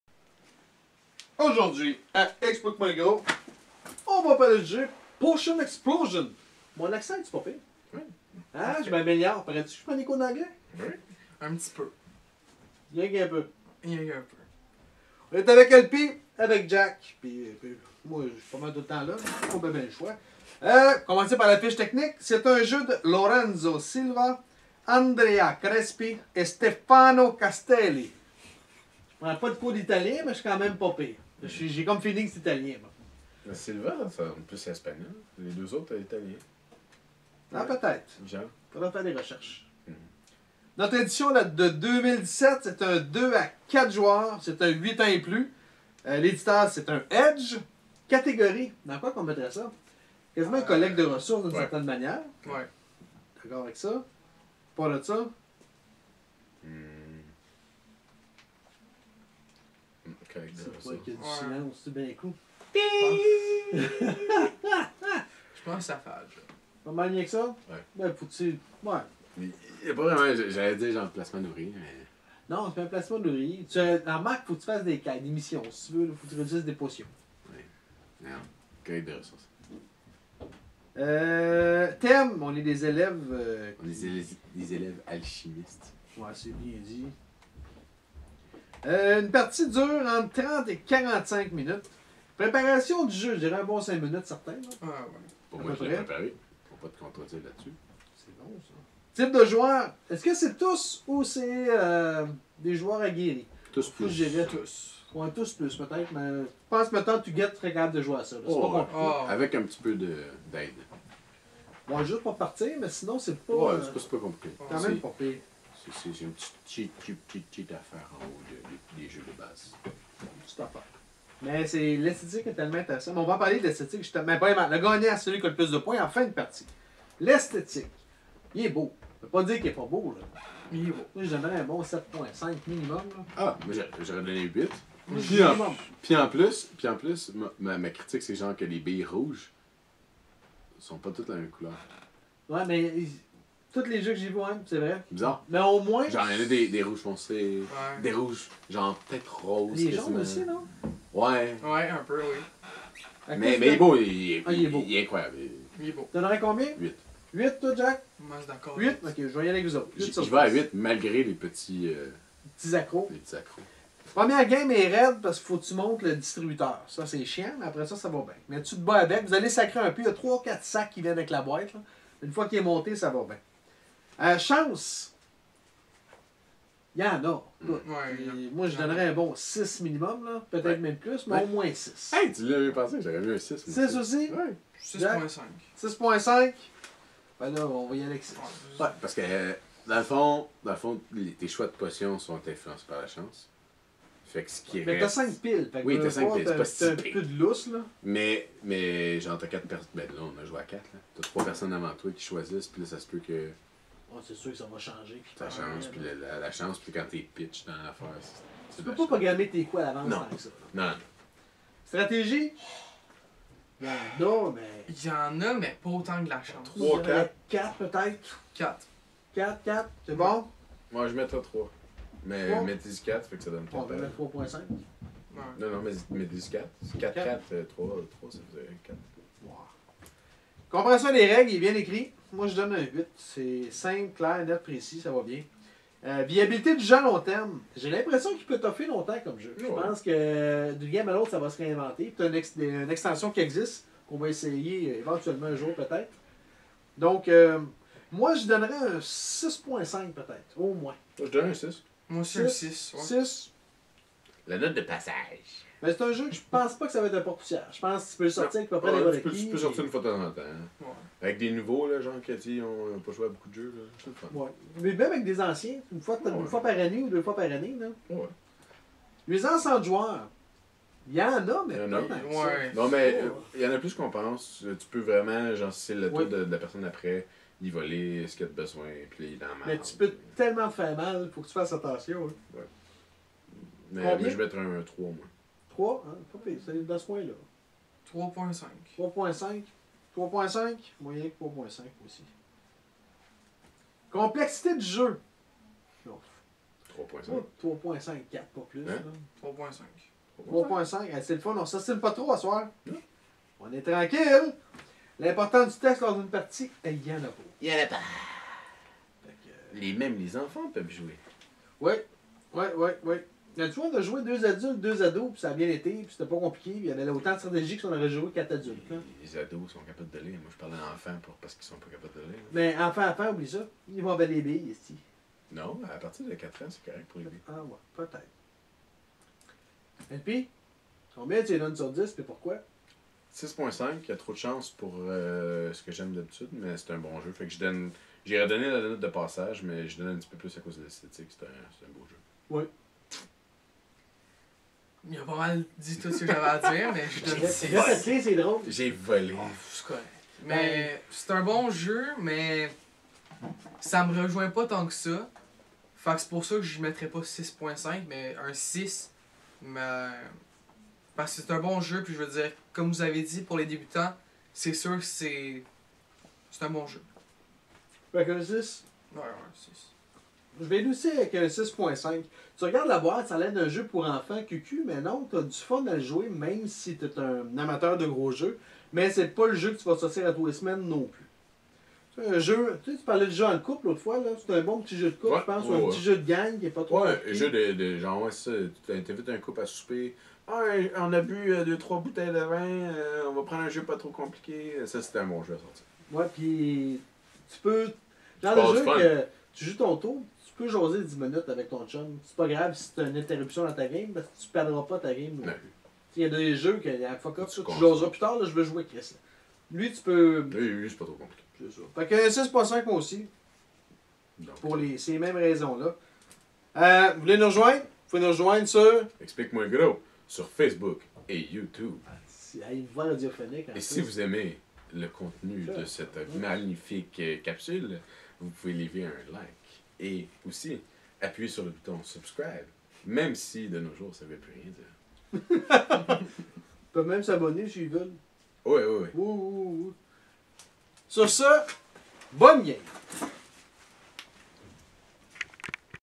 Potion explosion- Edge- 2017 -Critique
Le Gros, Jacob et L.-P. vous livrent une critique du jeu Potion explosion de  Lorenzo Silva, Andrea Crespi & Stefano Castelli édition de 2017 paru chez Edge. Ne vous gênez pas pour laisser la vôtre ...